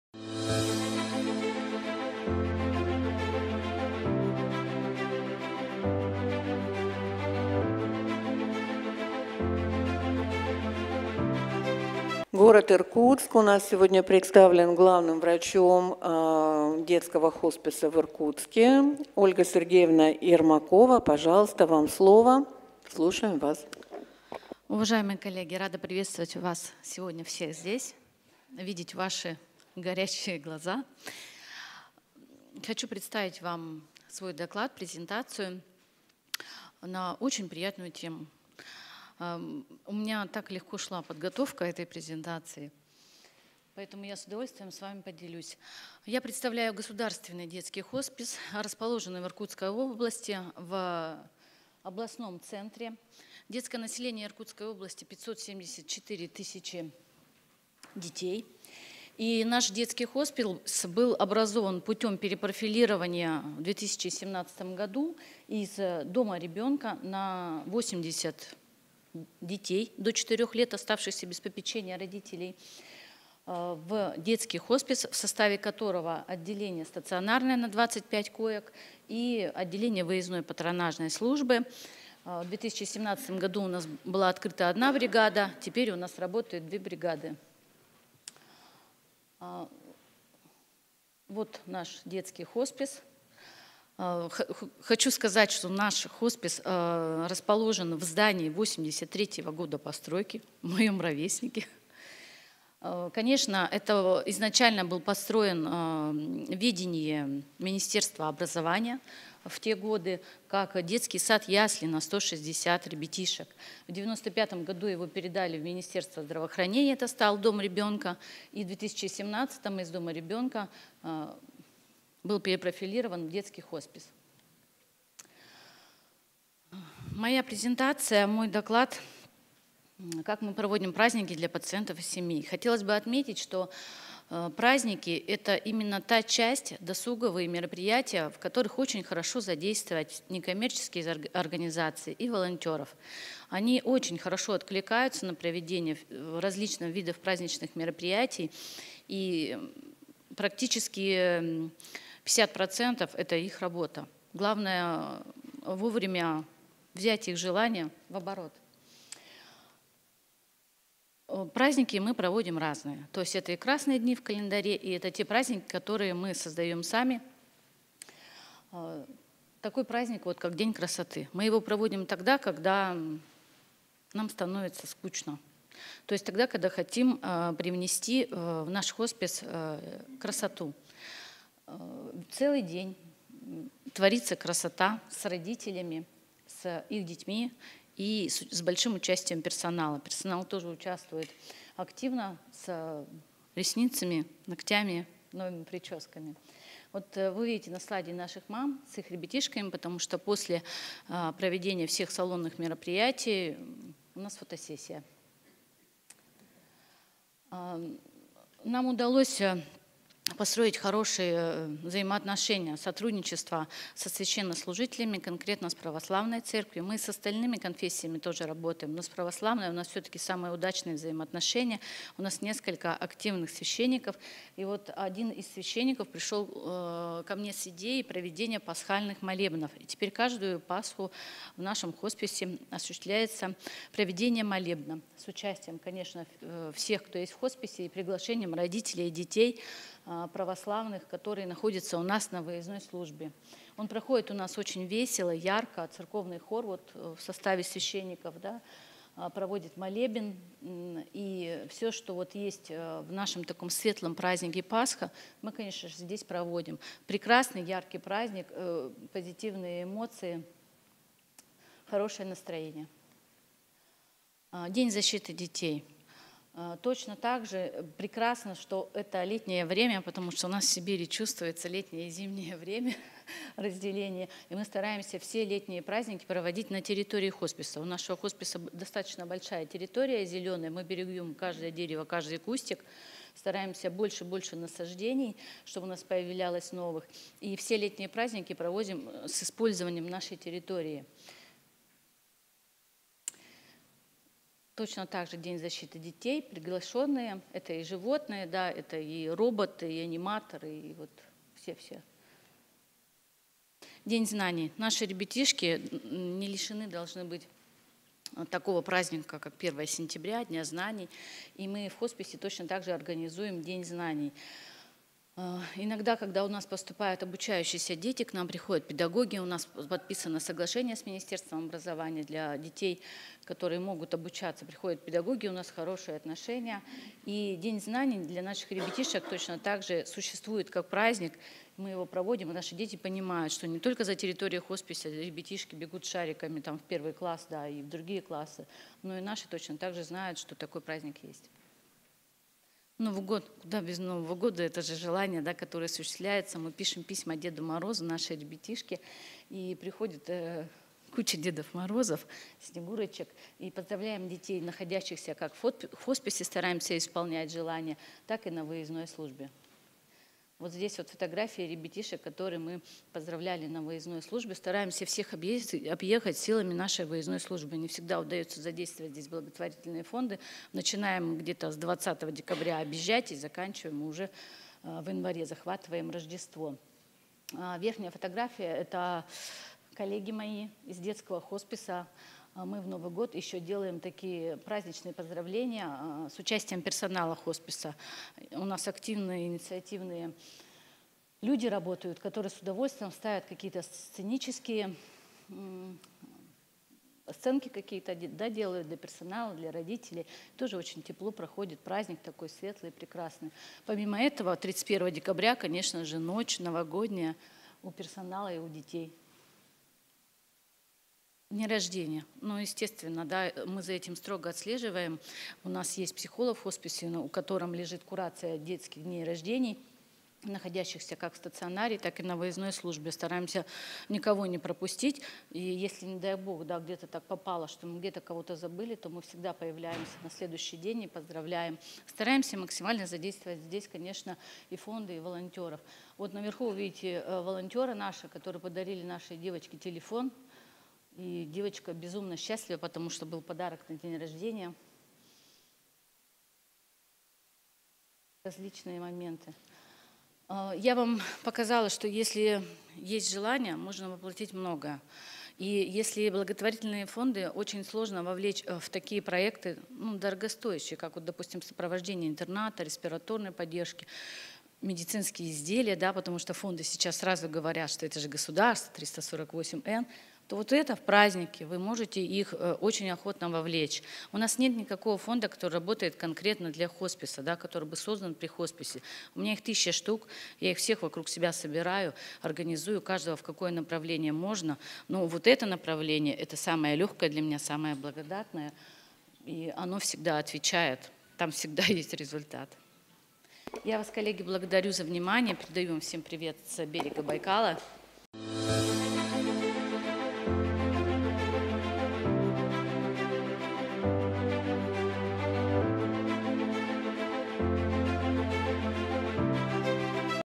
Как мы проводим праздники для пациентов и семей. Ермакова Ольга Сергеевна
► Доклад «Как мы проводим праздники для пациентов и семей»
߷ Ермакова Ольга Сергеевна, главный врач ОГБУЗ «Иркутский областной хоспис», главный внештатный детский специалист по паллиативной помощи Минздрава Иркутской области, главный внештатный детский специалист по паллиативной медицинской помощи Сибирского федерального округа (Иркутск)

⚑ VIII Ежегодная конференция с международным участием «Развитие паллиативной помощи взрослым и детям»
02-03 ноября 2022 года